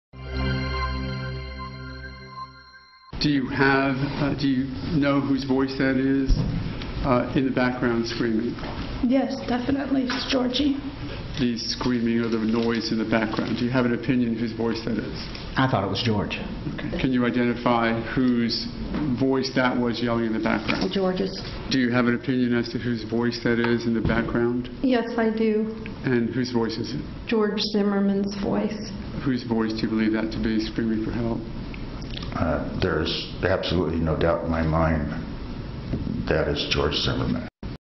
Video: 5 Zimmerman friends ID his voice on 911 call
Five of George Zimmerman's friends say they can identify his voice screaming in the background of a 911 call.